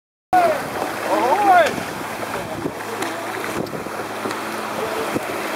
Pat's "1/2 Fast Cruiser" in Sarasota
I was taking pictures of the Everglades Challenge boaters in the Gulf of Mexico just outside the south end of Sarasota Bay and about a mile or 2 out. March of 2016